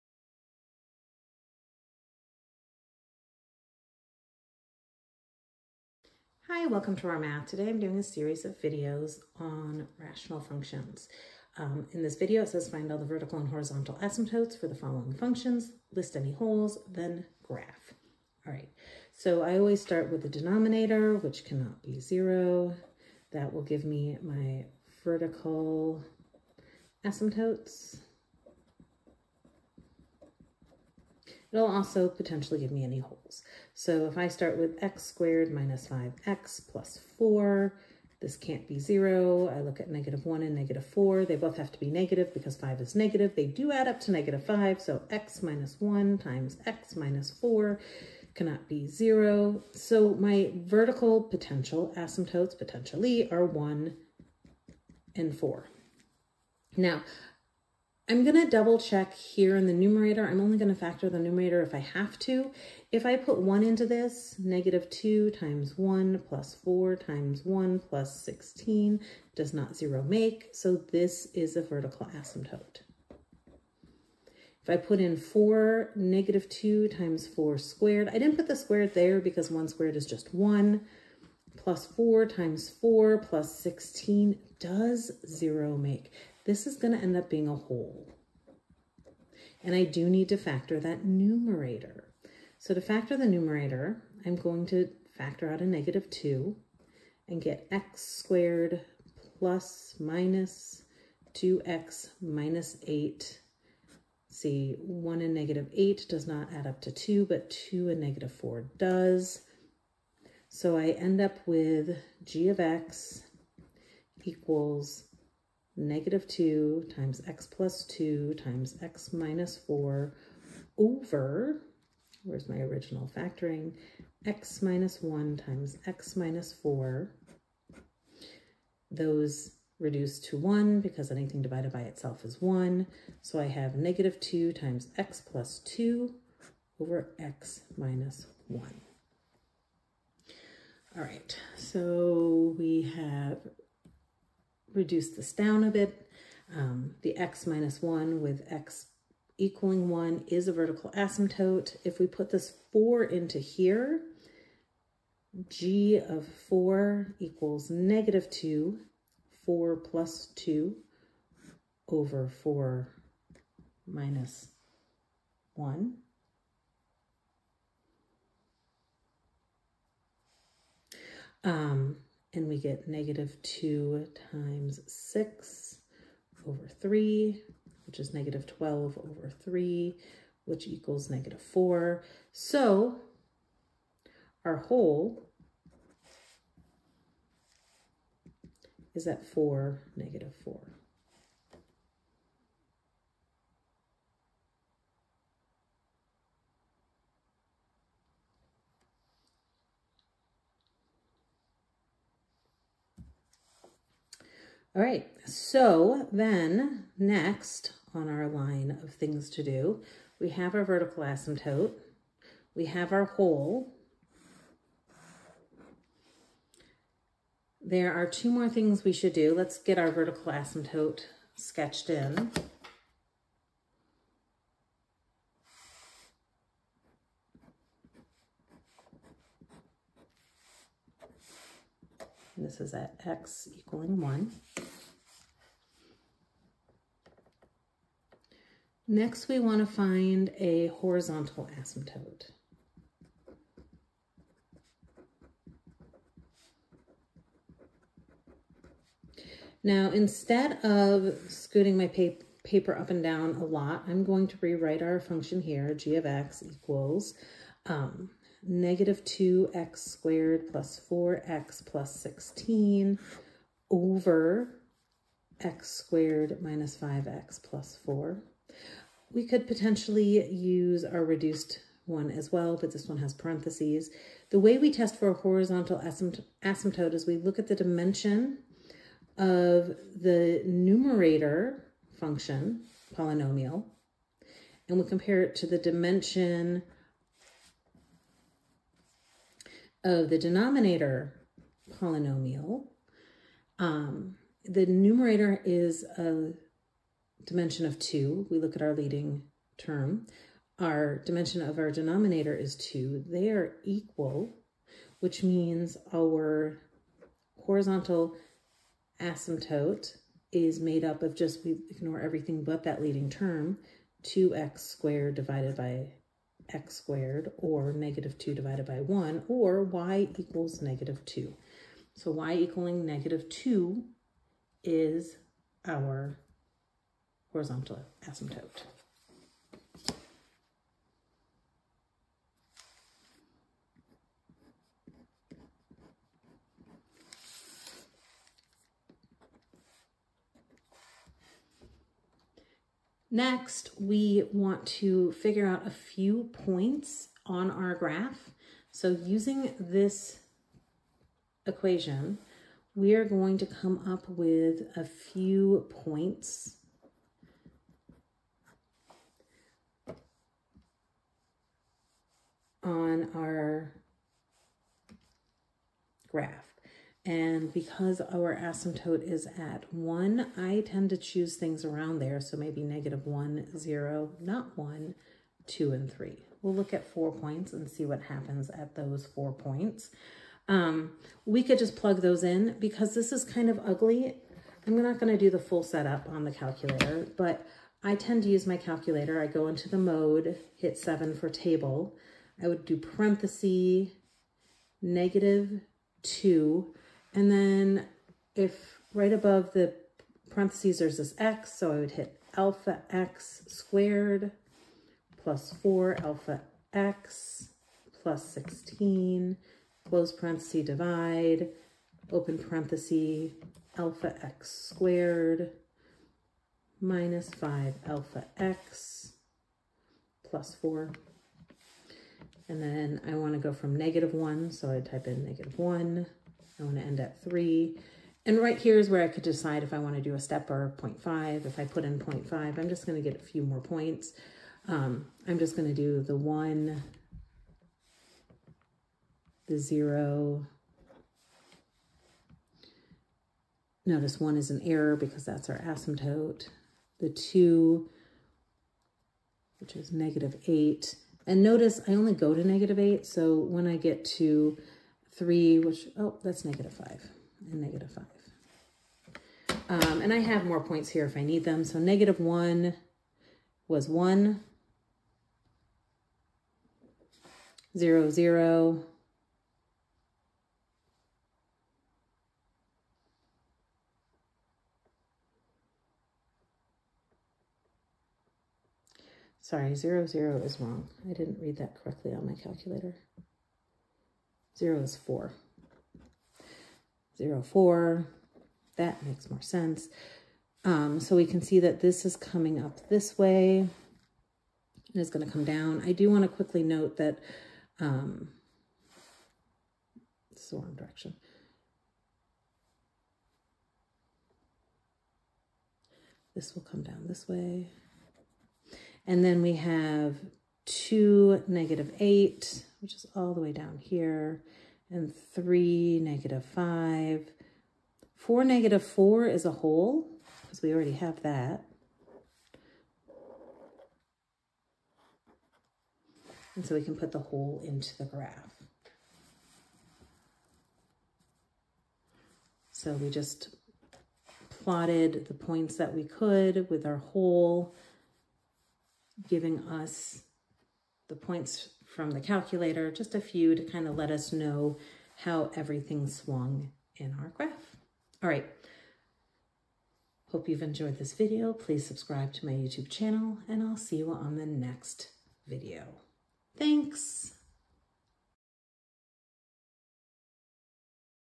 Rational Function - Asymptotes and Graphing
This video shows an example of graphing a rational function. There is a check for vertical and horizontal asymptotes and then a check of a few points to help define where the graph goes.